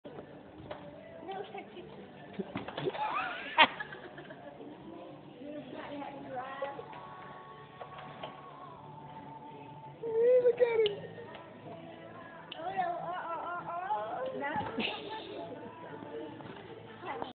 Jacob @ Toys R Us
Jacob riding through the toy store.